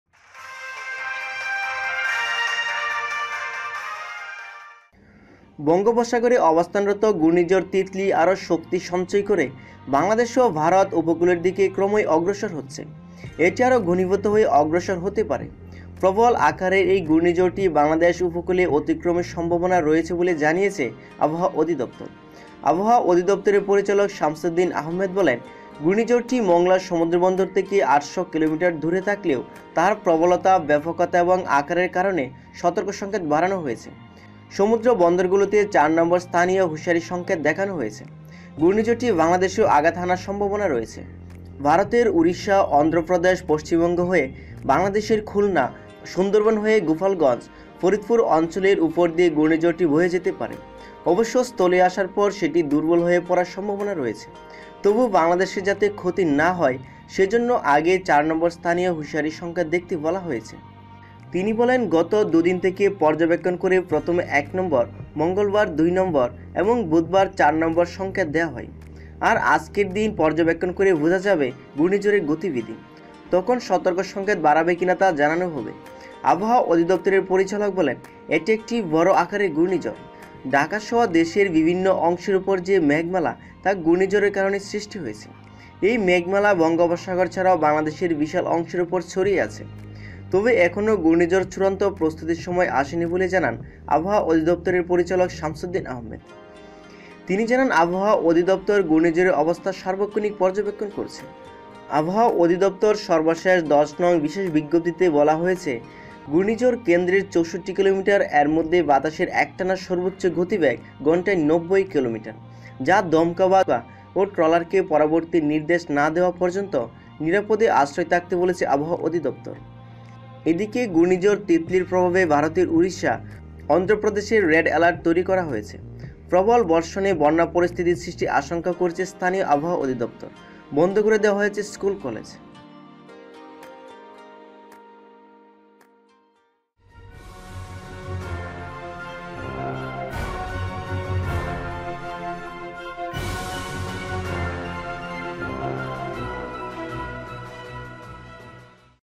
ভয়ানক দুঃসংবাদ ! ঘূর্ণিঝড় ‘তিতলি’ আঘাত হানতে পারে বাংলাদেশেও ! ঘূর্ণিঝড় ‘তিতলি শক্তিশালী রূপ নিল !
ঙ্গোপসাগরে অবস্থানরত ঘূর্ণিঝড় ‘তিতলি’ আরো শক্তি সঞ্চয় করে বাংলাদেশ ও ভারত উপকূলের দিকে ক্রমেই অগ্রসর হচ্ছে। এটি আরো ঘনীভূত হয়ে অগ্রসর হতে পারে। প্রবল আকারের এই ঘূর্ণিঝড়টি বাংলাদেশ উপকূল অতিক্রমের সম্ভাবনা রয়েছে বলে জানিয়েছে আবহাওয়া অধিদফতর।

আবহাওয়া অধিদফতরের পরিচালক সামসুদ্দিন আহমেদ বুধবার (১০ অক্টোবর) সকালে বাংলানিউজকে বলেন, ঘূর্ণিঝড়টি (রিপোর্টের সময় অবস্থান) মোংলা সমুদ্র বন্দর থেকে ৮০০ কিলোমিটার দূরে থাকলেও তার প্রবলতা, ব্যাপকতা এবং আকারের কারণে সতর্ক সংকেত বাড়ানো হয়েছে। সমুদ্র বন্দরগুলোতে ৪ নম্বর স্থানীয় হুঁশিয়ারি সংকেত দেখানো হয়েছে।

ঘূর্ণিঝড়টি বাংলাদেশেও আঘাত হানার সম্ভাবনা রয়েছে জানিয়ে তিনি বলেন, ১১ অক্টোবর (বৃহস্পতিবার) রাতে বা ১২ অক্টোবর (শুক্রবার) ভোরে ঘূর্ণিঝড়টি বাংলাদেশ উপকূল অতিক্রম করতে পারে।

‘ভারতের উড়িষ্যা-অন্ধ্রপ্রদেশ-পশ্চিমবঙ্গ হয়ে বাংলাদেশের খুলনা, সুন্দরবন হয়ে গোপালগঞ্জ, ফরিদপুর অঞ্চলের উপর দিয়ে ঘূর্ণিঝড়টি বয়ে যেতে পারে। অবশ্য স্থলে আসার পর সেটি দুর্বল হয়ে পড়ার সম্ভাবনা রয়েছে। তবুও মানুষের যাতে ক্ষতি না হয় সেজন্য আগেই ৪ নম্বর স্থানীয় হুঁশিয়ারি সংকেত দেখাতে বলা হয়েছে।’

তিনি বলেন, গত দু’দিন থেকে পর্যবেক্ষণ করে প্রথমে ১ নম্বর, মঙ্গলবার ২ নম্বর এবং বুধবার ৪ নম্বর সংকেত দেওয়া হয়। আর আজকের দিন পর্যবেক্ষণ করে বোঝা যাবে ঘূর্ণিঝড়ের গতিবিধি। তখন সতর্ক সংকেত বাড়বে কিনা তা জানানো হবে।